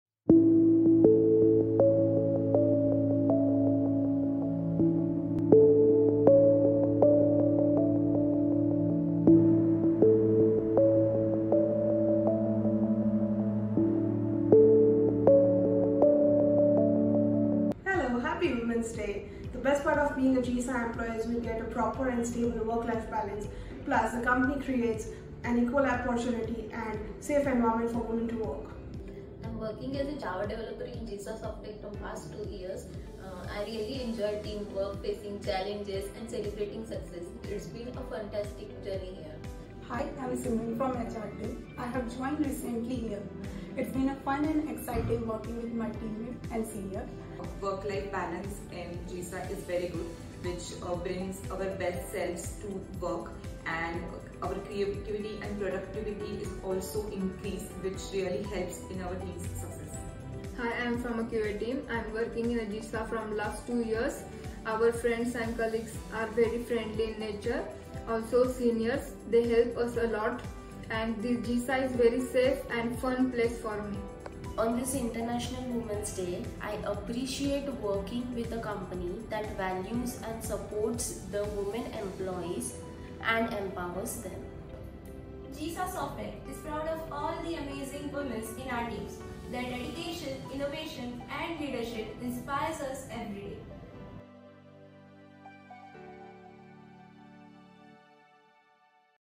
Women In Tech
Celebrating International Women's Day with a spotlight on the incredible Women in Tech at JISA! Join us in honoring the achievements and contributions of the talented women who are shaping the future of technology.

We had the incredible opportunity to speak with some truly inspiring women at JISA to hear their stories, challenges and experiences. Their insights are inspiring and their journeys are empowering.